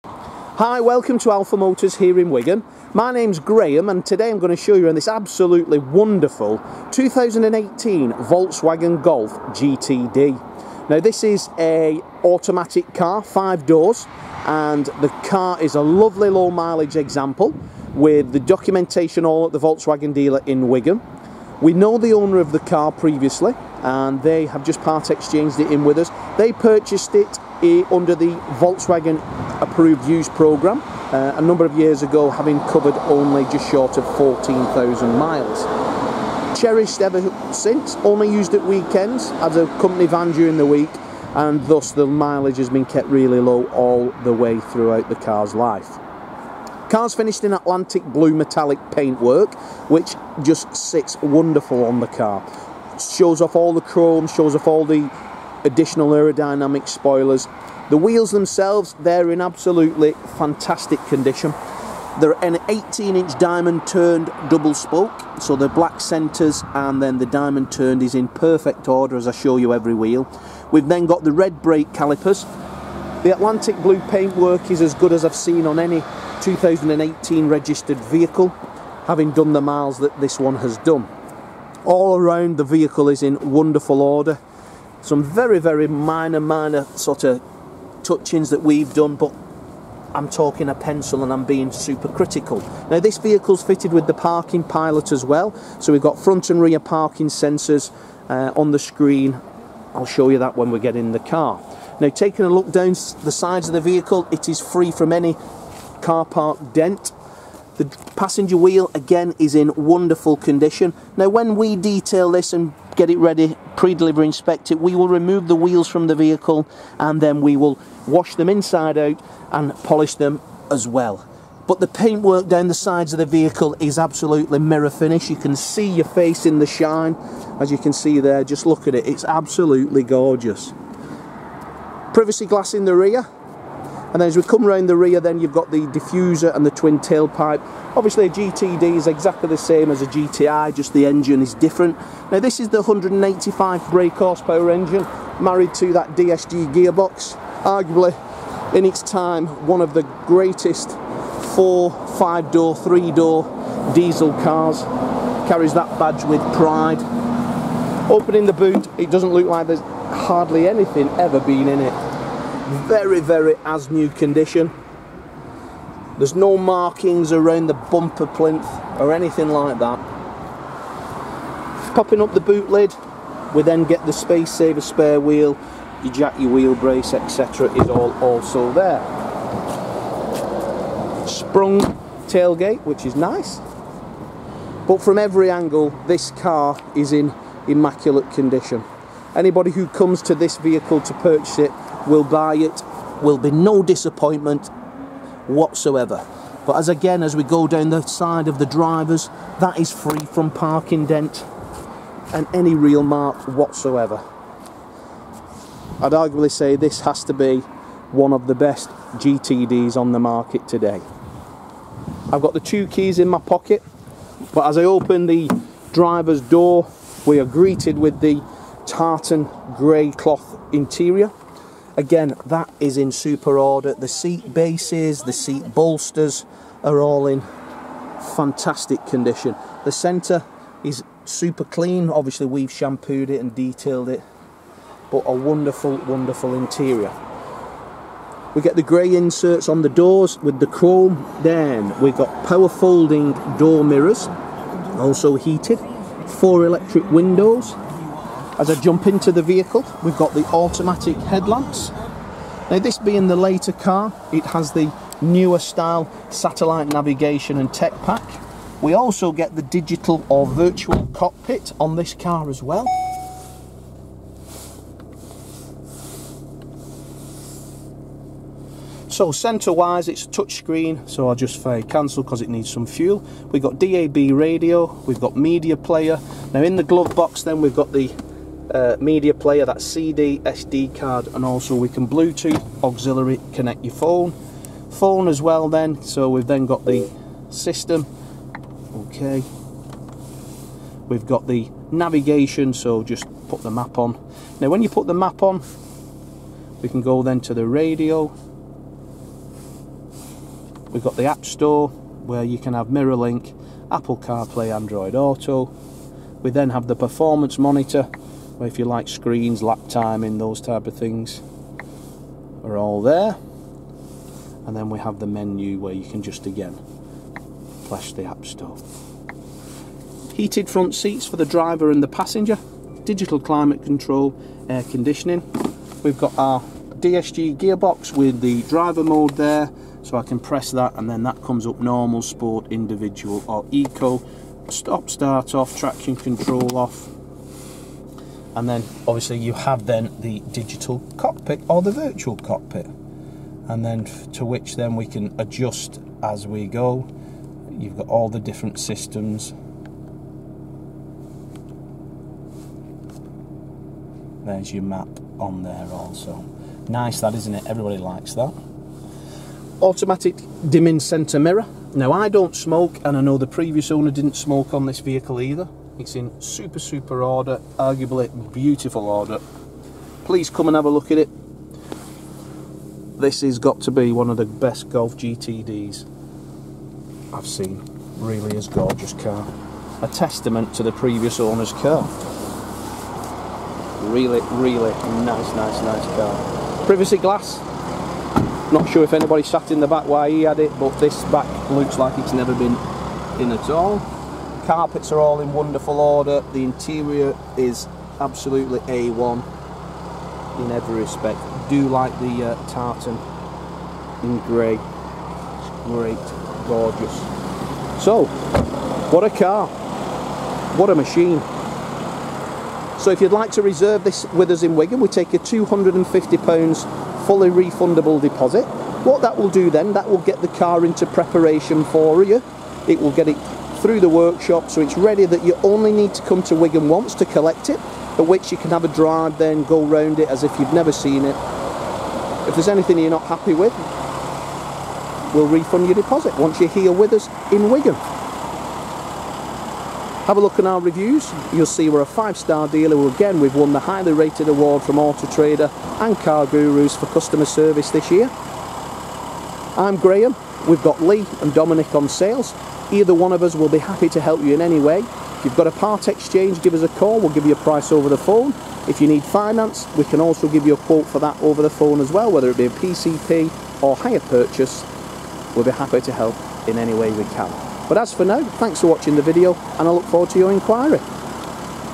Volkswagen Golf 2.0L GTD TDI DSG
Alpha Motors a family-owned award-winning business for over 40 years in Wigan are proud to present this VW Golf GTD TDi 185 Auto.
Presented in stunning Atlantic Blue metallic paintwork and sits on 18-inch alloys.
Inside boasts a Black-Grey Tartan check interior.

Purchased from the Volkswagen Main Dealer by the previous keeper in August 2020 having covered 13705 miles. A beautiful example throughout that must be viewed!

++FACTORY OPTIONS++Technology Pack++Atlantic Blue Metallic paint++Privacy glass++VW Virtual Cockpit ++

Great spec car with SAT NAV, Cruise Control-Speed limiter, Digital Climate Control, Multi Function Leather Steering Wheel, Bluetooth, Heated power folding door mirrors, Voice Command, Auto Lights, 18-inch alloys, Privacy Glass, VW Park Pilot, VW Multi-Media, Heated seats, Drive Mode Select, plus much more!

SERVICE HISTORY: service @ 13705 VW, 23779 VW.
Professionally prepared to the highest standards and presented with 2 keys, HPI clear report, and book pack.
Call today to register your interest!